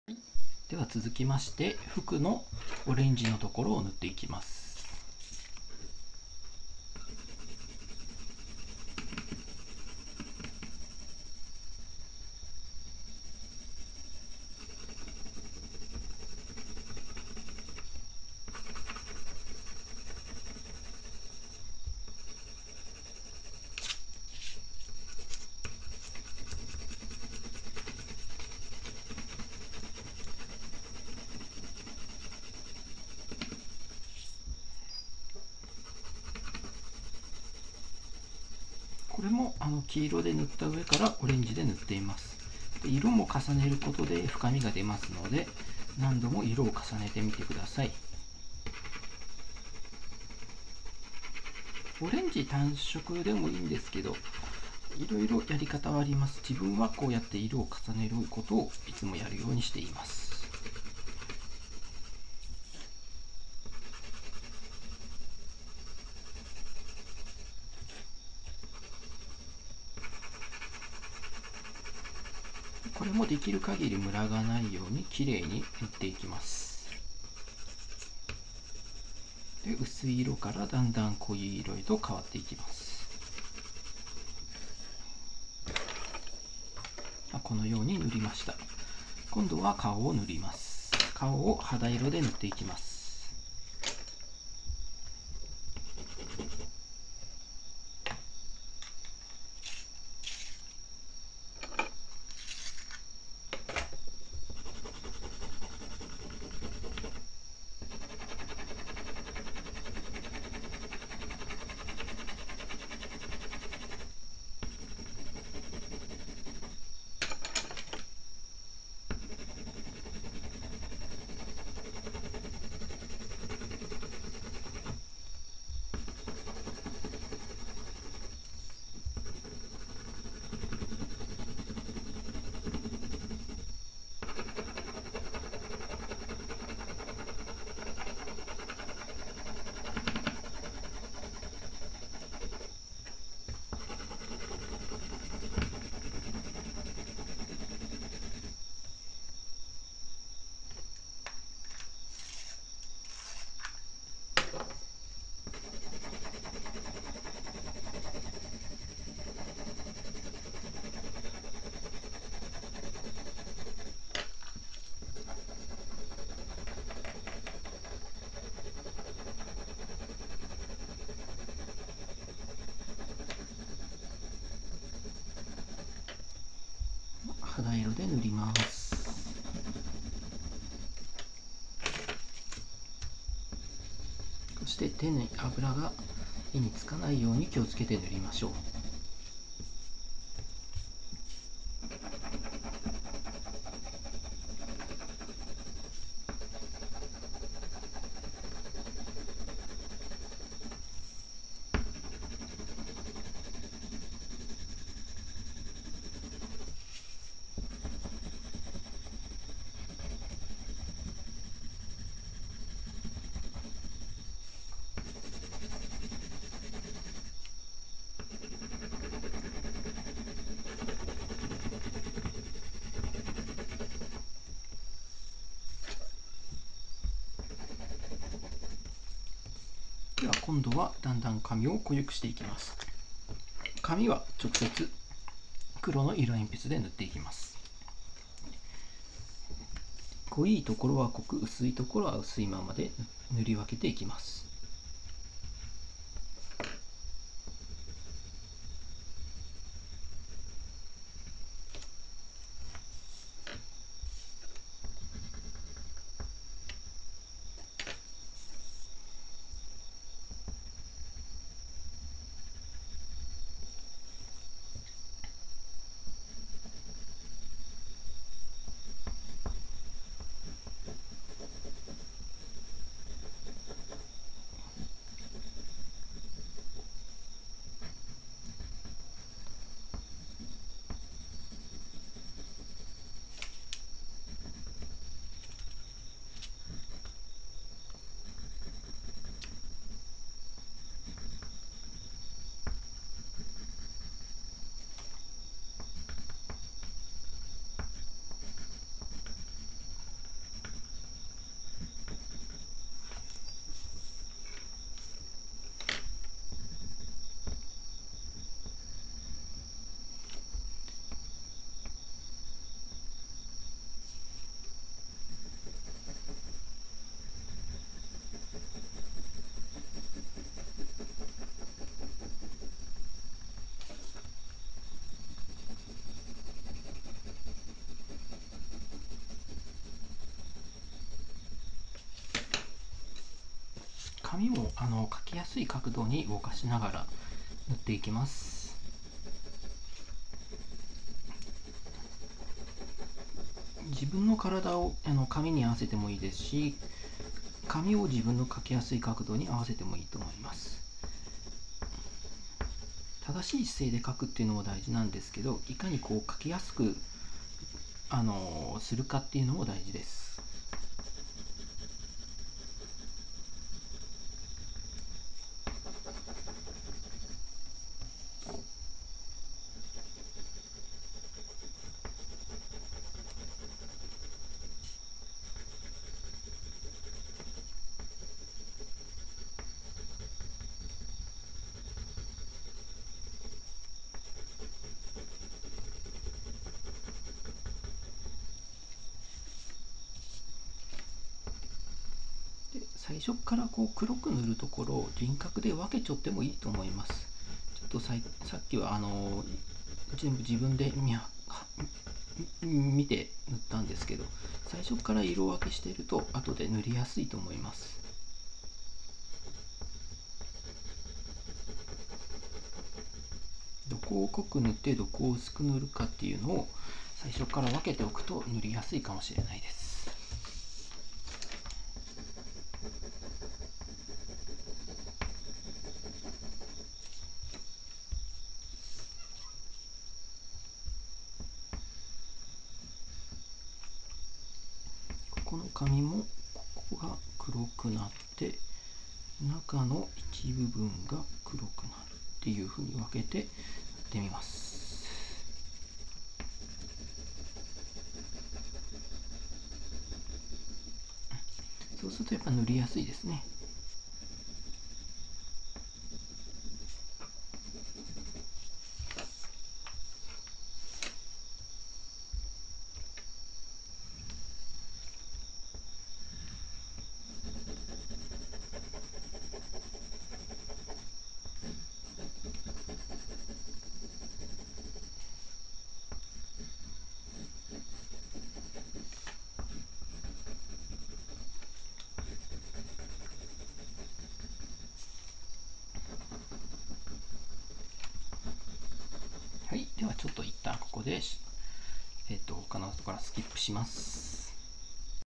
④ ドラゴンボールの悟空を描こう
ドラゴンボールの悟空の色塗りを説明しております😀
色鉛筆は、色を何度も重ねて塗ってください☺
ムラがないように丁寧に塗ってみてください。